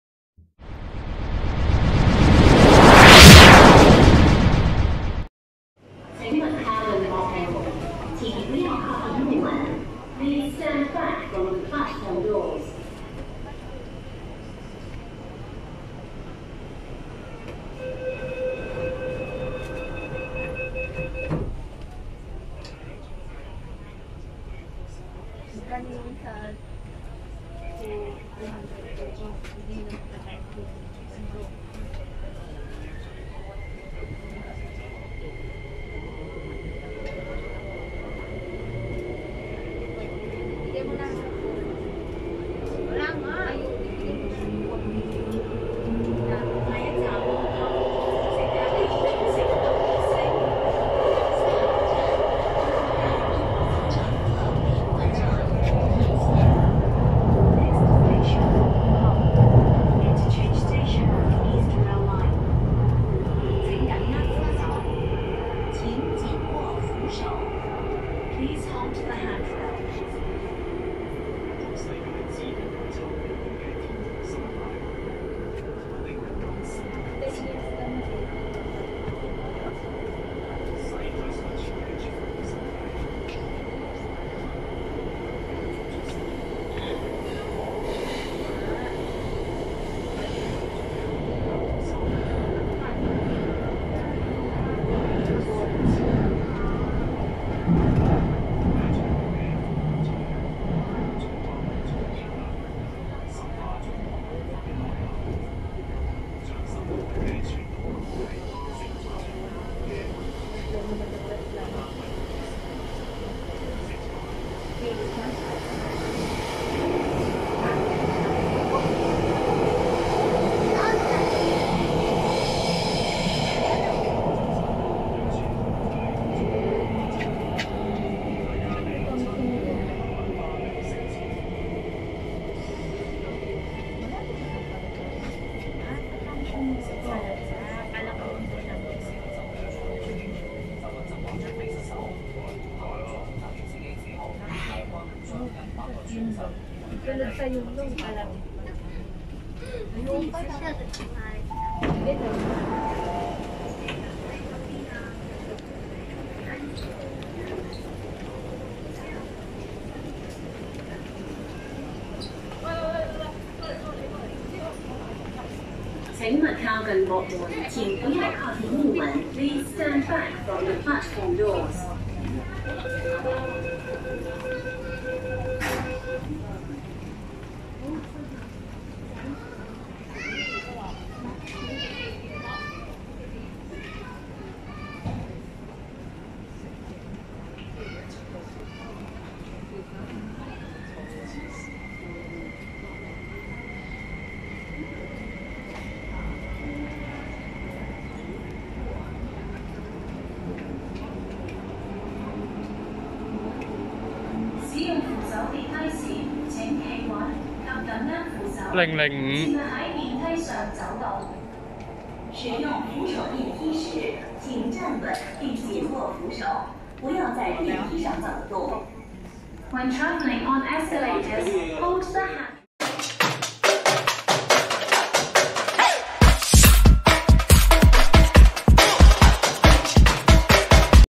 [屯馬線短途行車回程中咗2次Webull廣告車]TML IKK-Train(D347/D348)尖東-紅磡
拍攝於:2022年7月3日(星期日)
使用功能製作片段:FlimMaker
列車:屯馬線IKK-Train(D347/D348)，車序0
全列車編組:D347-P347-M347-H347+C348-M348-P348-D348
目的地:尖東站起點至紅磡終點站
被楚屯馬線回程嘅時候居然又中咗2次Webull廣告車，今次係唔同D347/D348